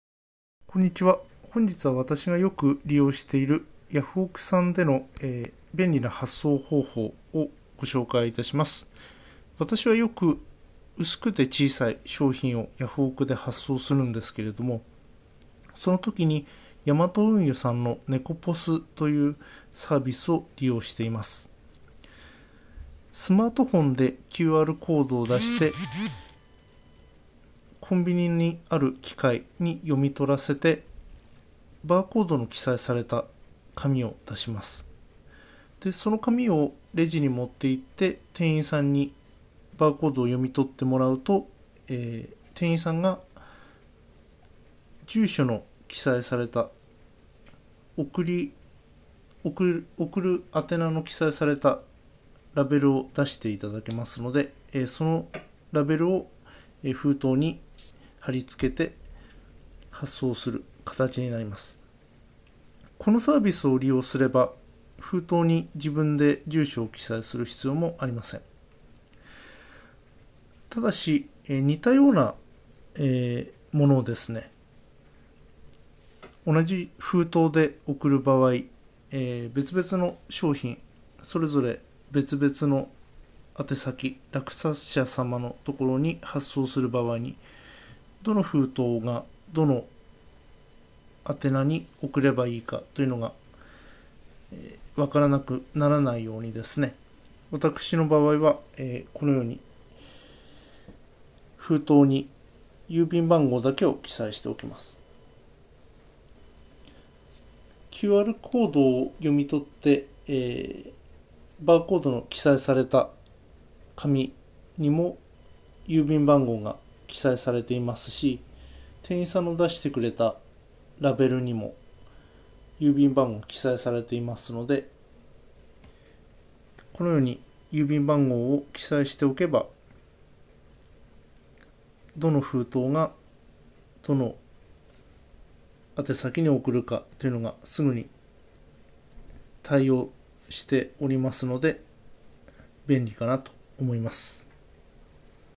ヤフオクさんの大量発送で薄く小さな物にネコポスが便利♪
ヤマト運輸さんのネコポスは、A4サイズ以下の大きさ、2.5㎝以下の厚さ、1㎏以下の重さ限定です。